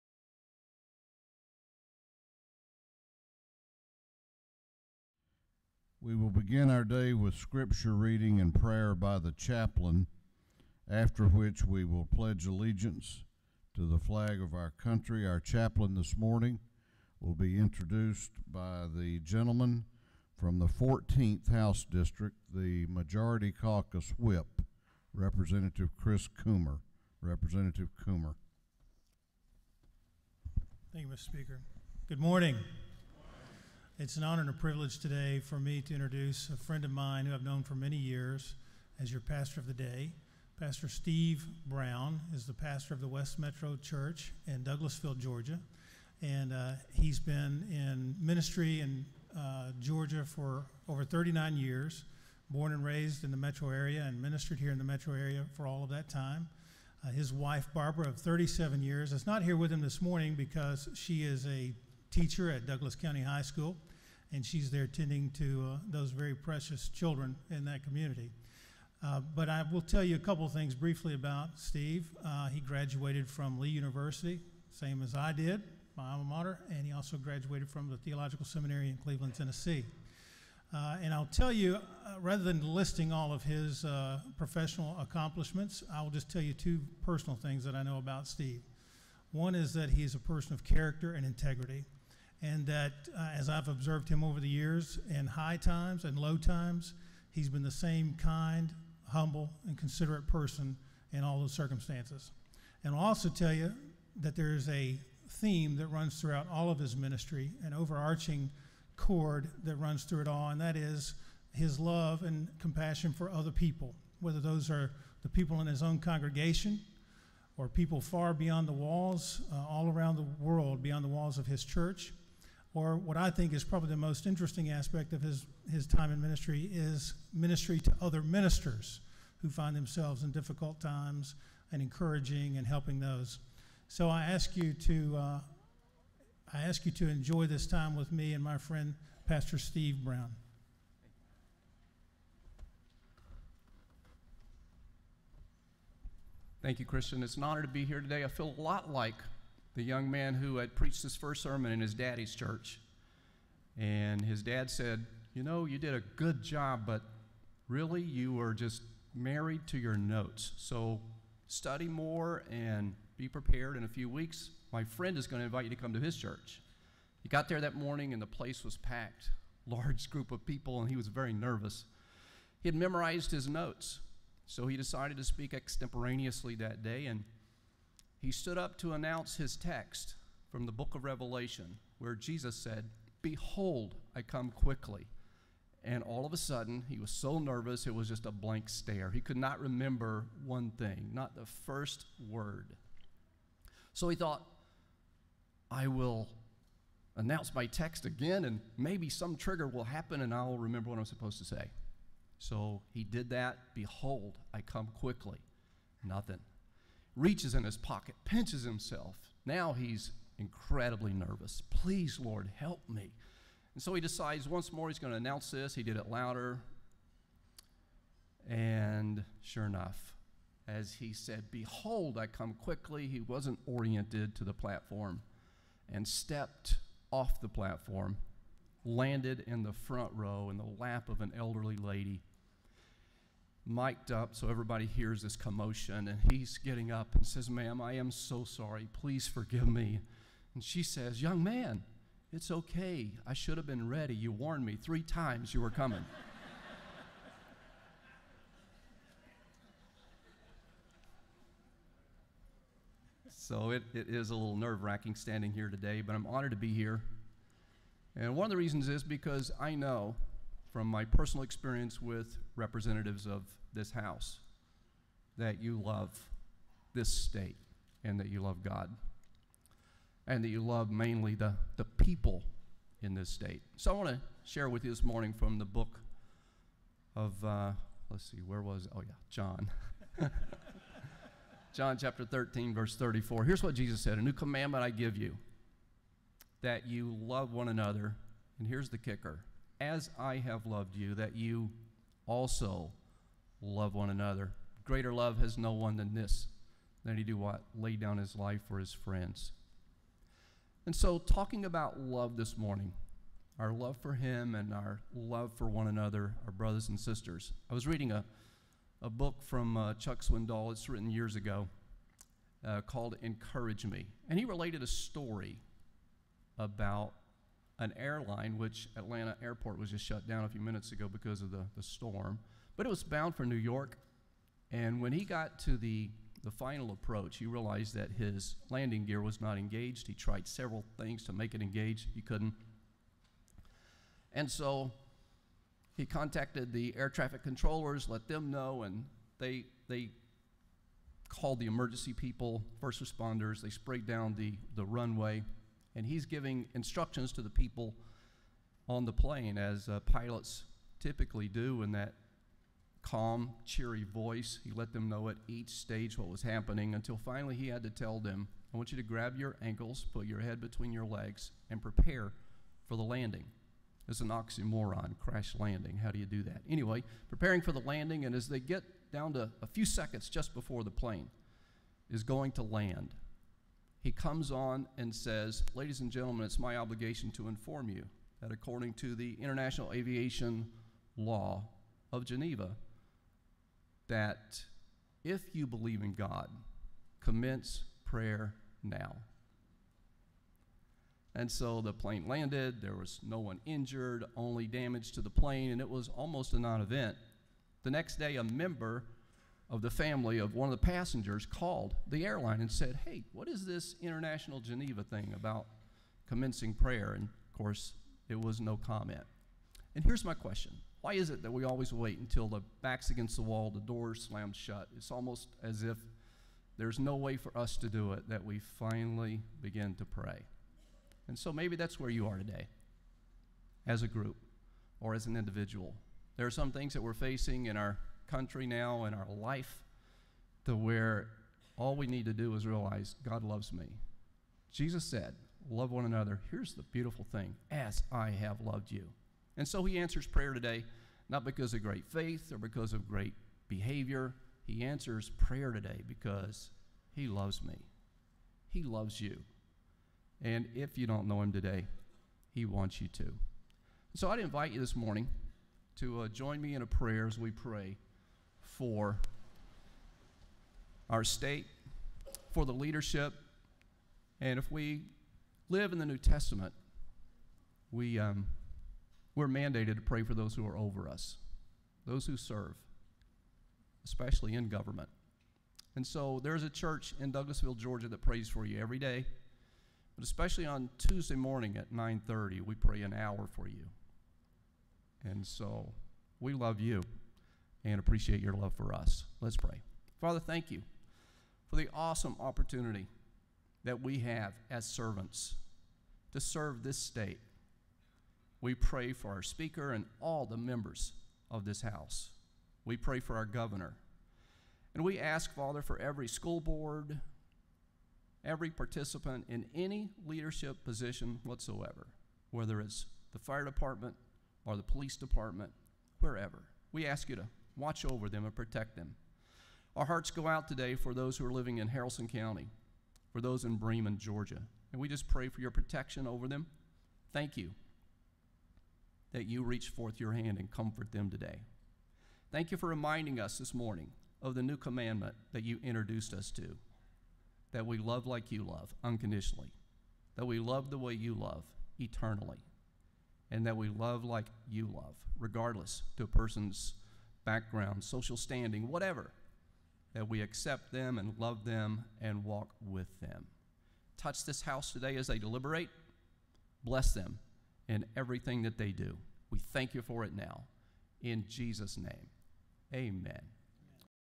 Day 17: Chaplain of the Day
Rev. Steve Brown introduced by Majority Whip Christian Coomer